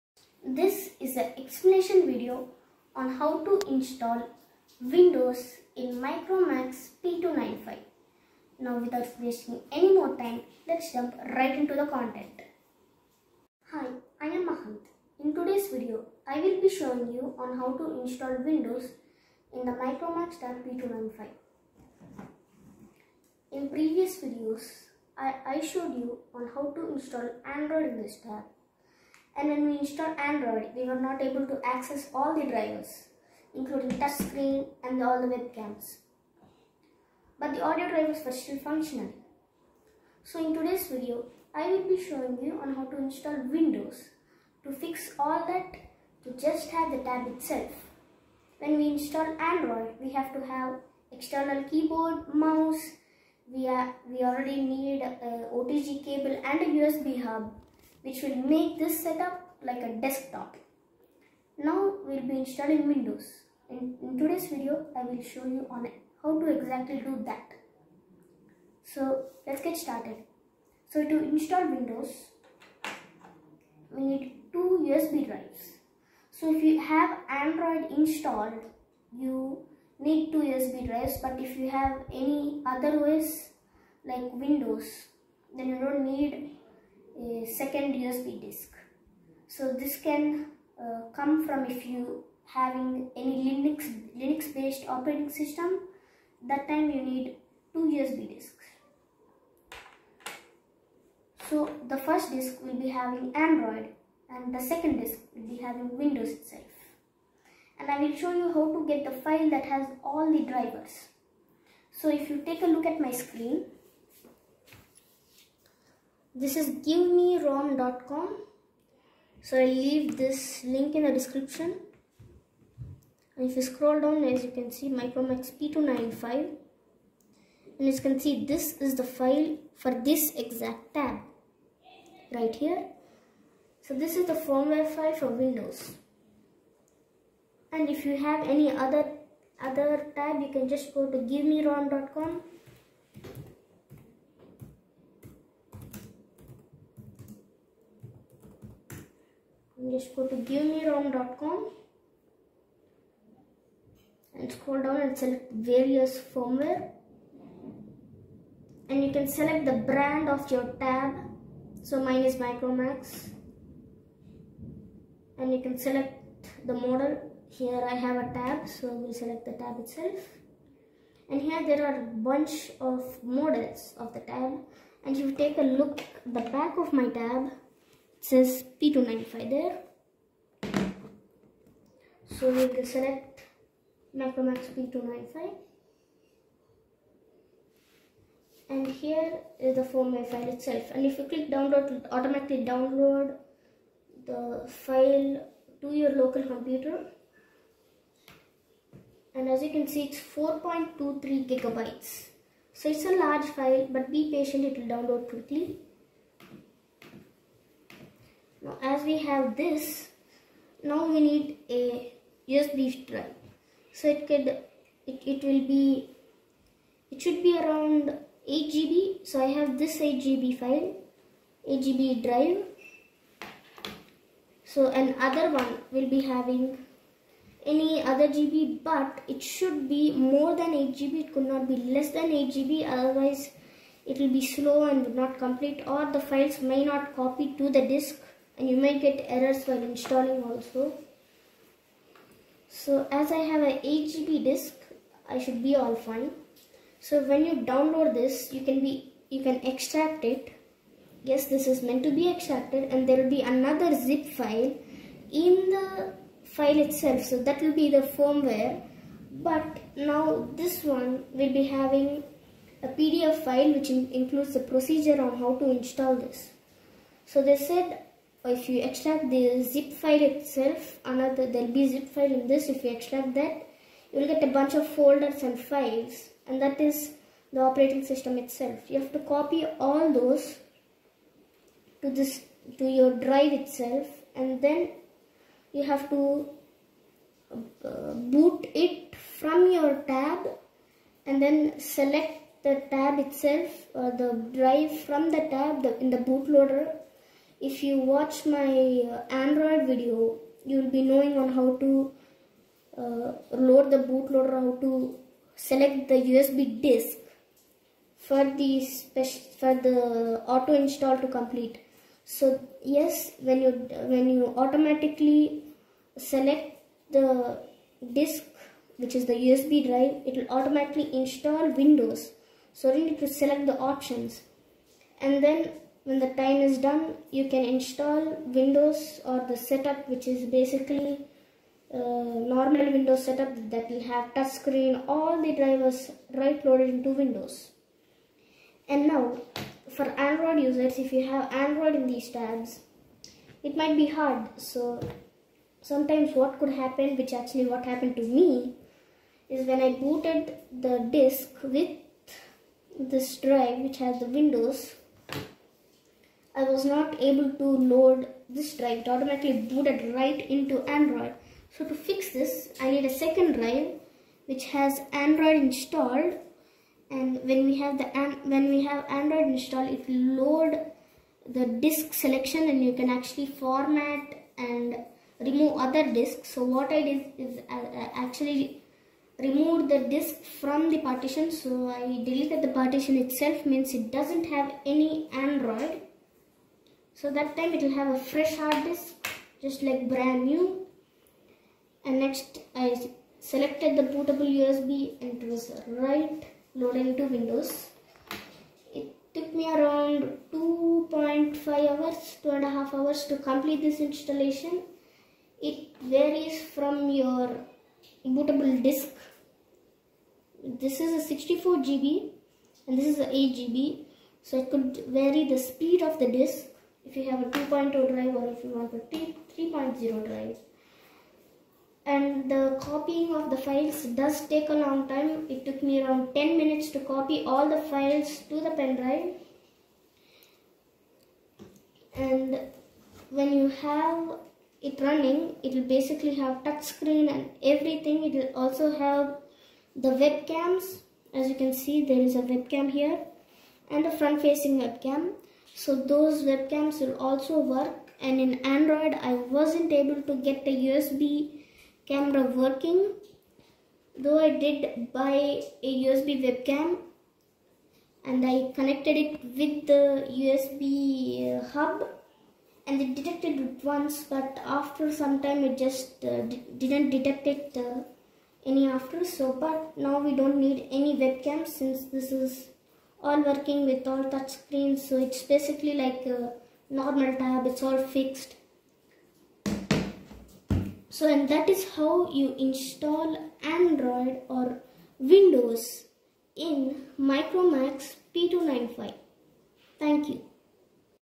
Explanation On How To Install Windows 10 in Micromax P295 Tablet
This video shows on how to install windows 10 in micromax p295.